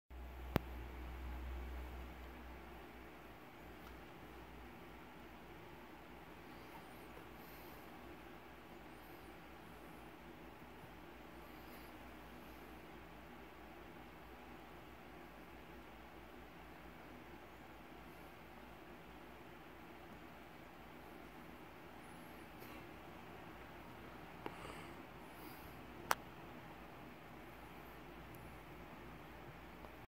Puppy dreaming shaking
3 month old German Shepherd half pitbull